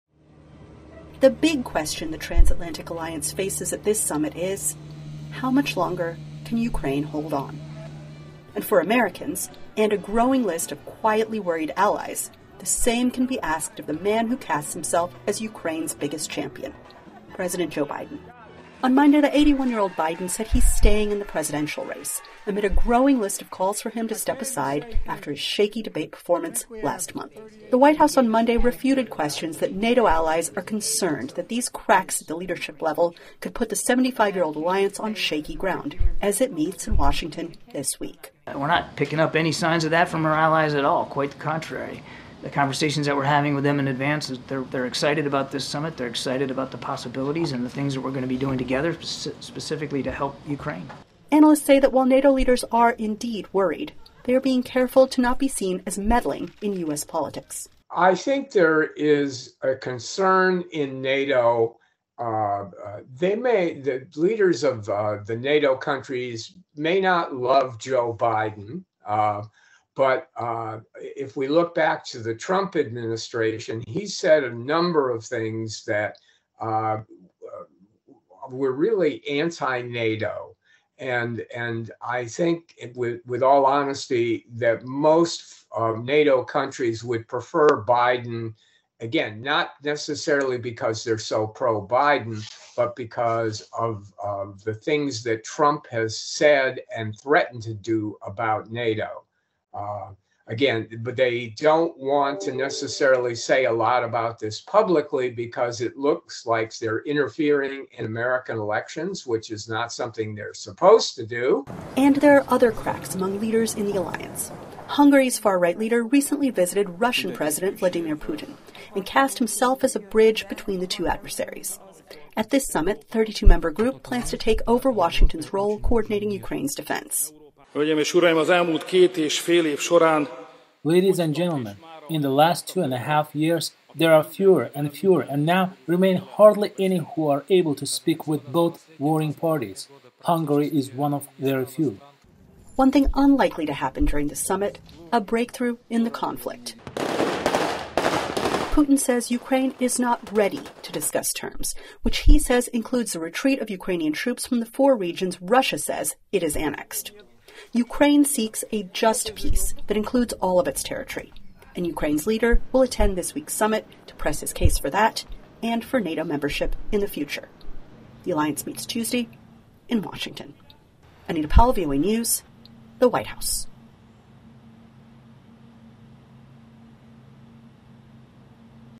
NATO alliance meets under cloud over President Biden’s future | VOANews
President Joe Biden welcomes members of the newly enlarged NATO alliance this week for a summit aimed at planning for Ukraine’s future defense — and, some observers say, “Trump-proofing” it if Biden loses the November poll amid growing doubts over his future.